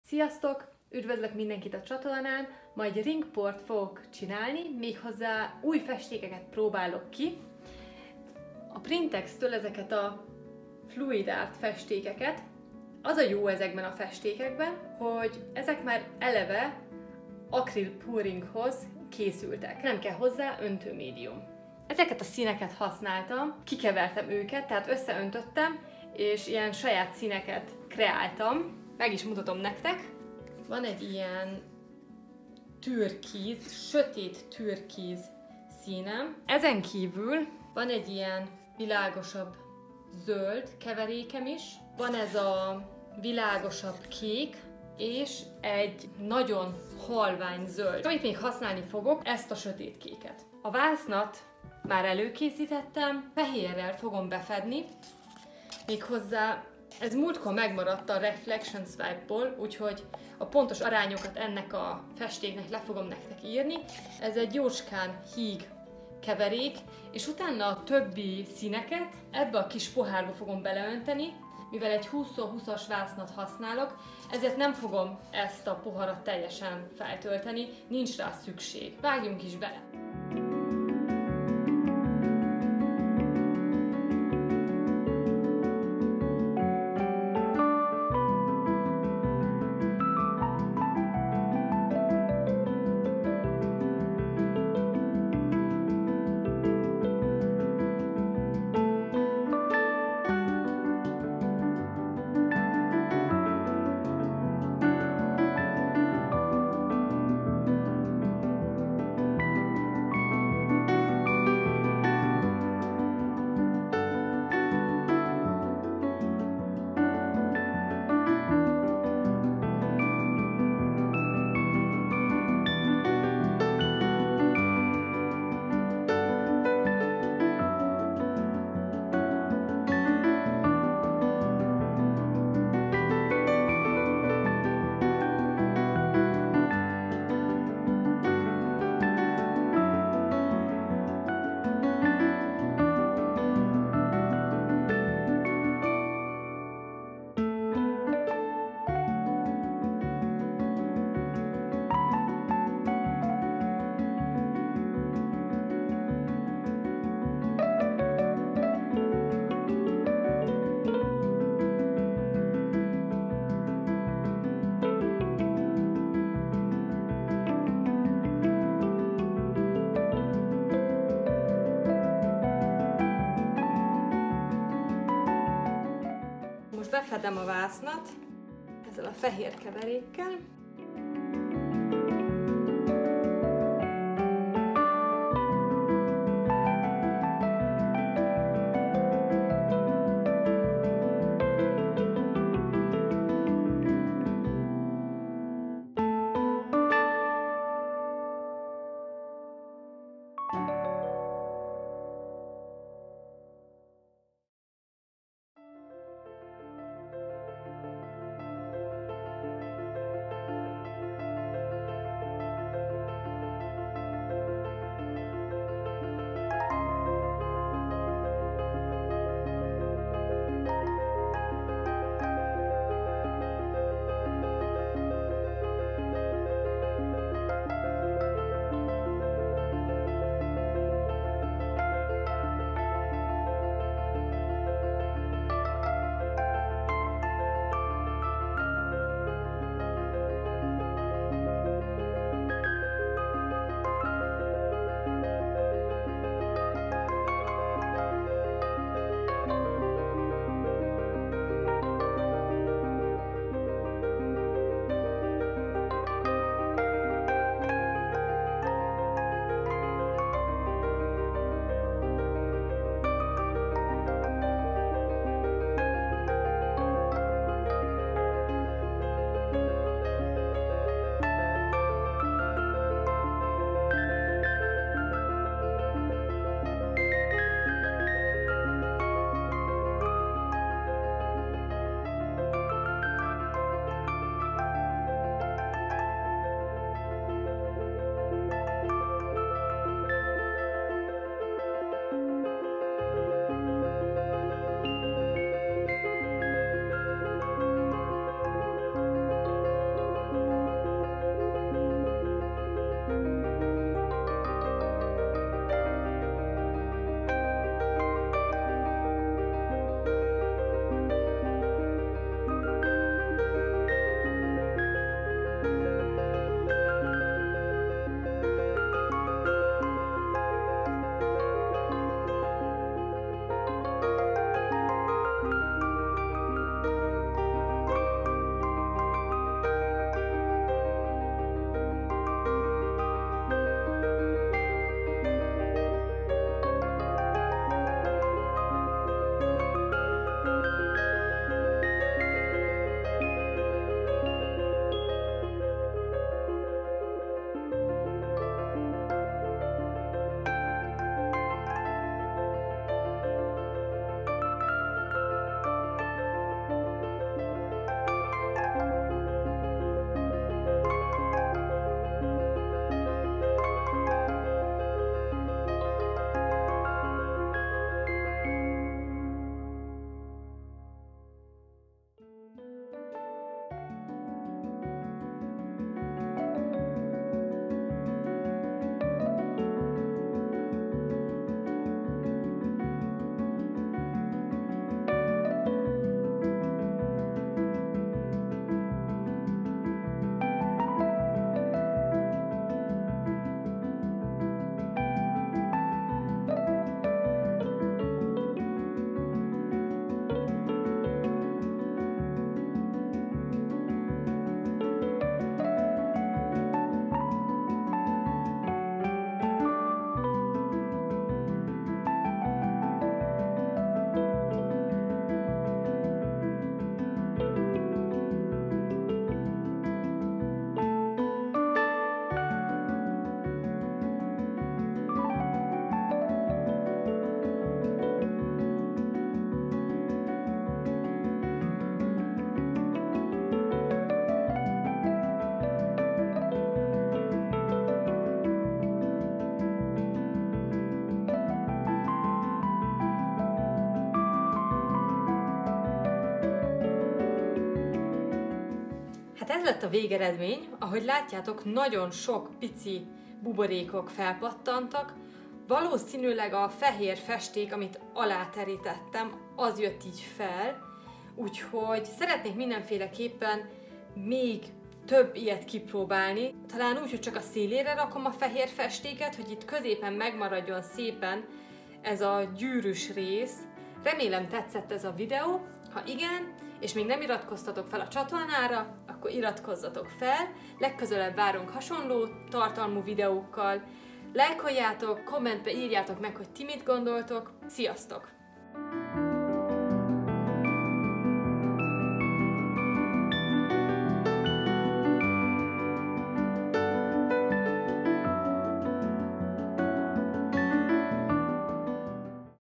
Kék Ring Pour | Akril Pouring #47
Sziasztok! Máté és Eszti vagyunk. 🙂
A fluid art printex akril pouring festékekkel készítünk egy ring pour-t.
Ezek a festékek a weboldalon elérhetőek.

A fehér festékhez a recept:
4 rész Owatrol Floetrol
1 rész Amsterdam Titanium White
+ víz, hogy jócskán híg legyen

Ezt a képet meg tudod venni.